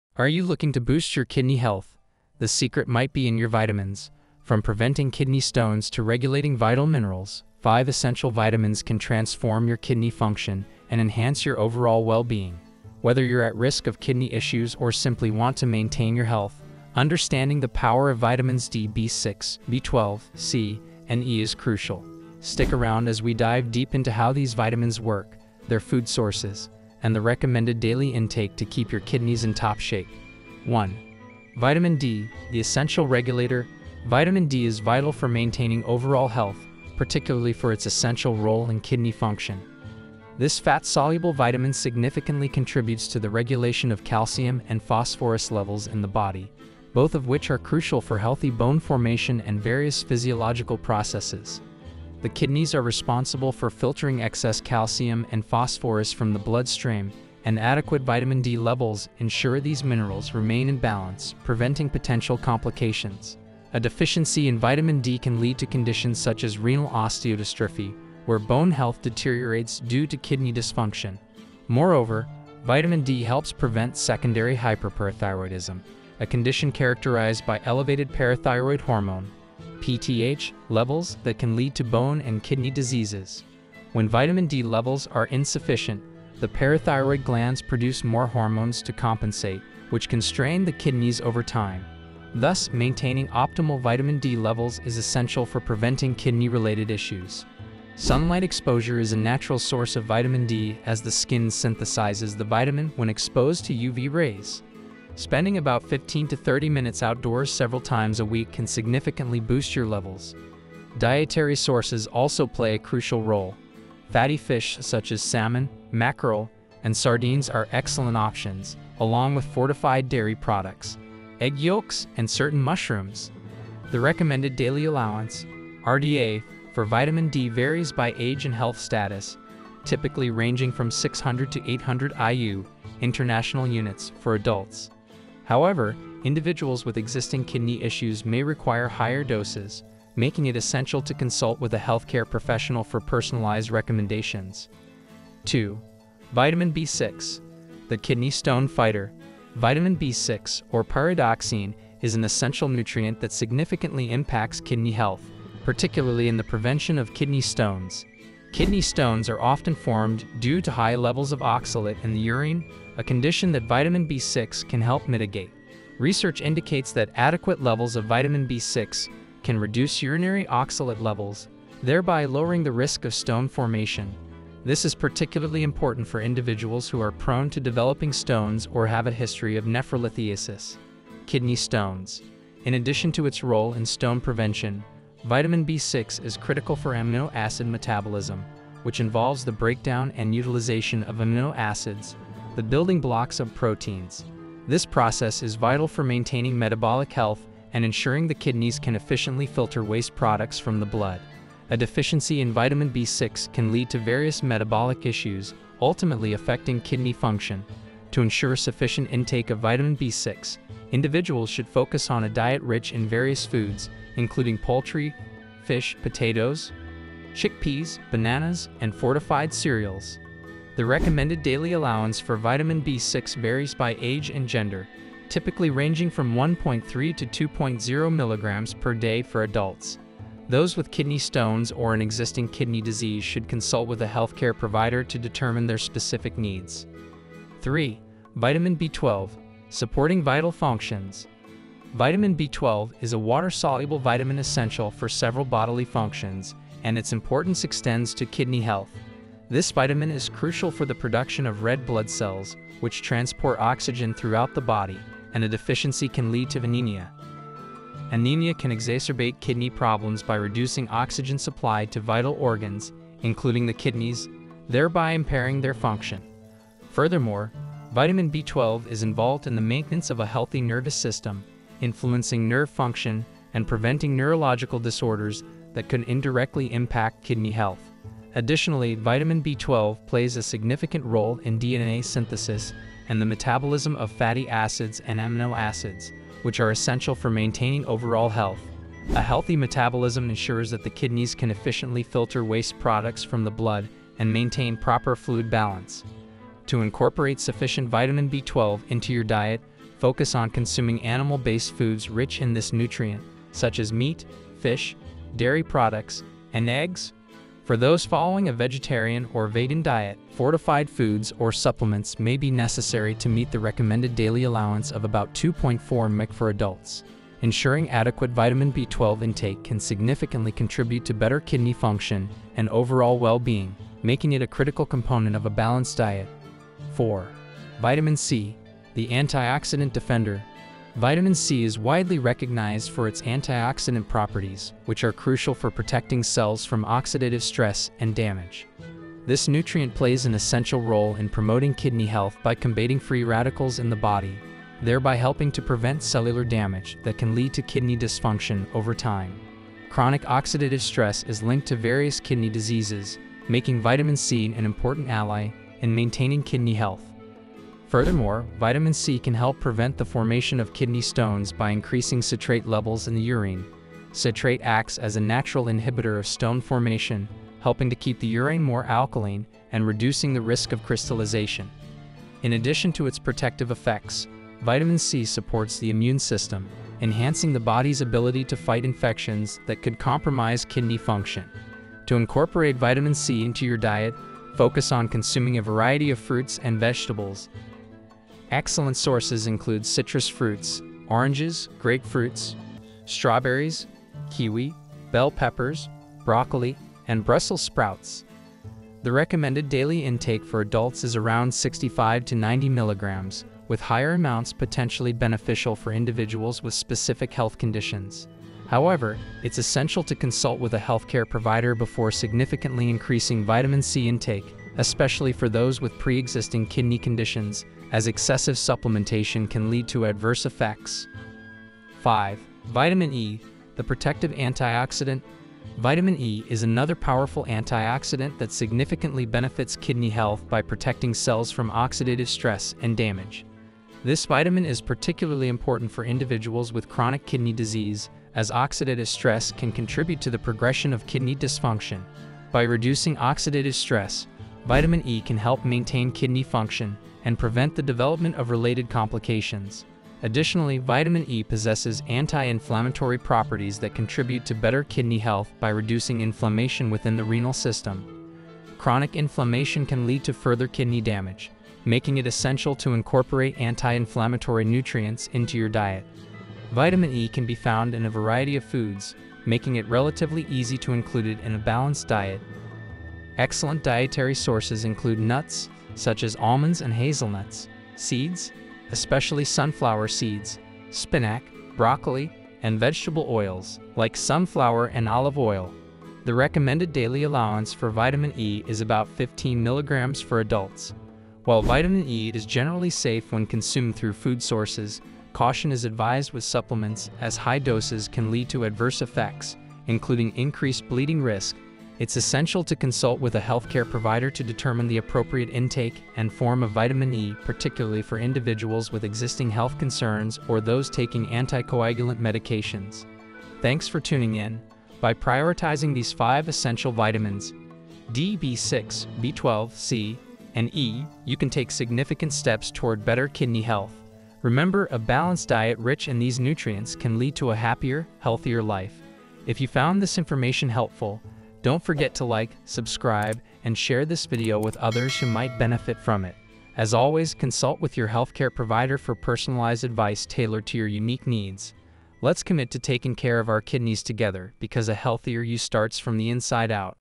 Just 5 Vitamins You Need For Better Kidney Function
In this video, we discuss the top 5 essential vitamins that can help improve kidney function naturally. Learn about the benefits of each vitamin and how they can support your overall kidney health. By incorporating these vitamins into your daily routine, you can take proactive steps towards maintaining healthy kidneys. Watch now to discover the key vitamins for better kidney function!